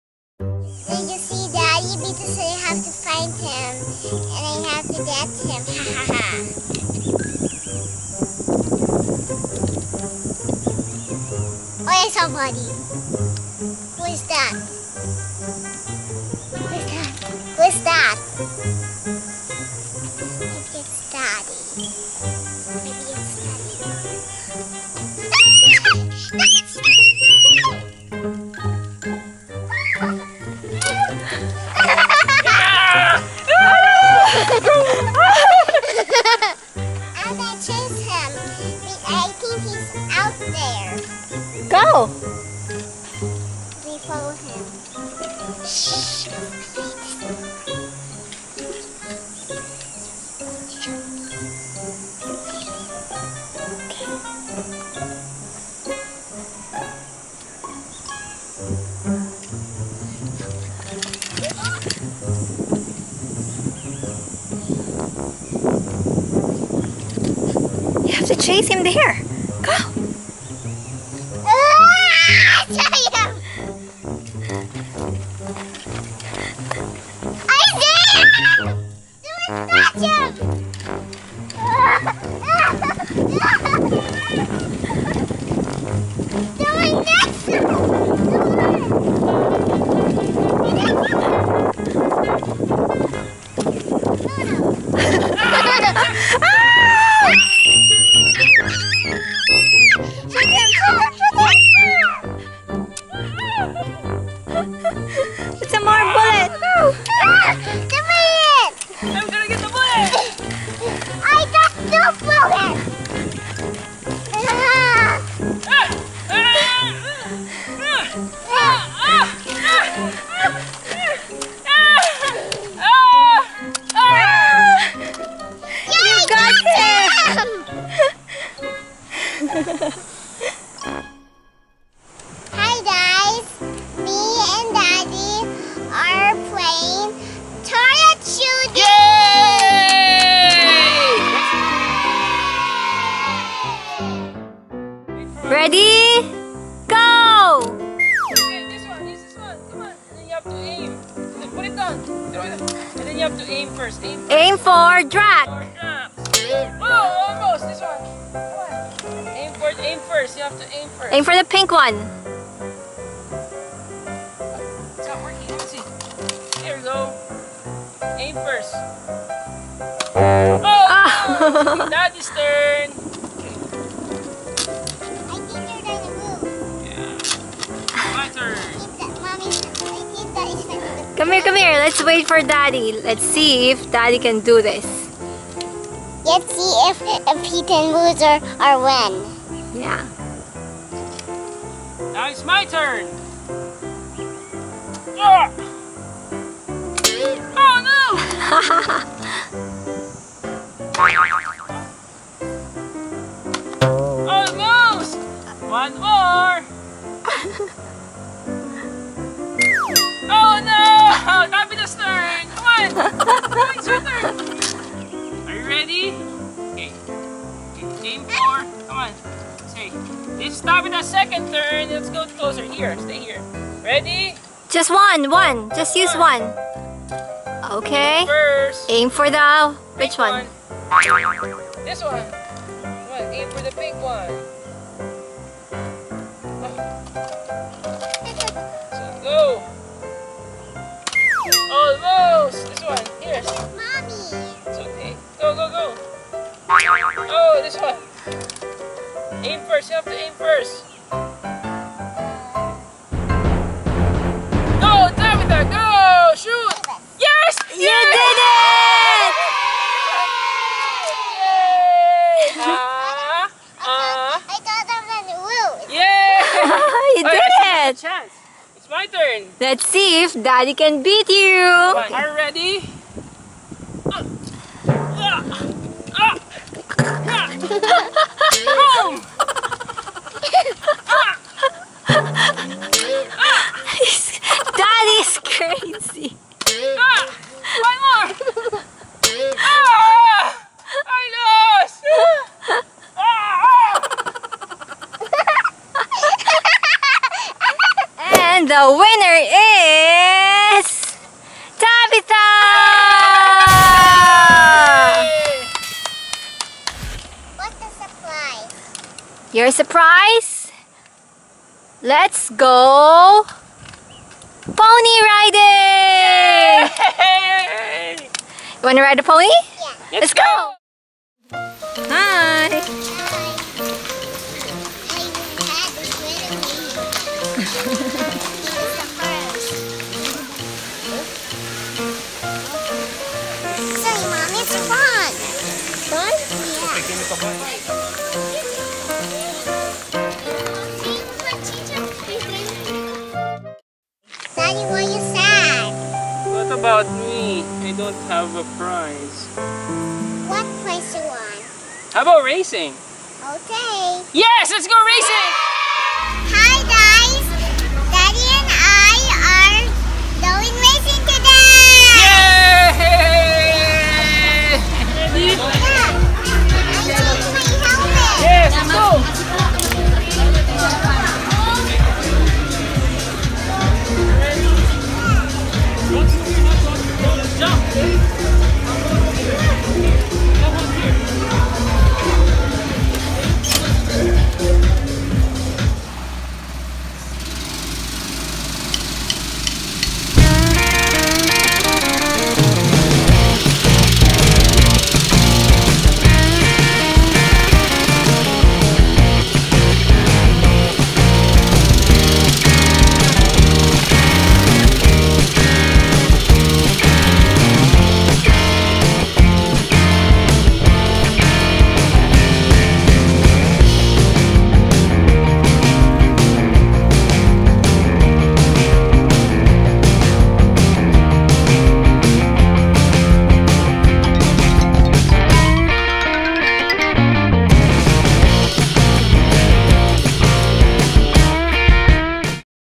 Tabby Vlogs: Fun Games Pony Ride & Go Kart Race | Baby Playful
Tabby and Daddy do fun games, then she rides a cute pony, and they go kart racing!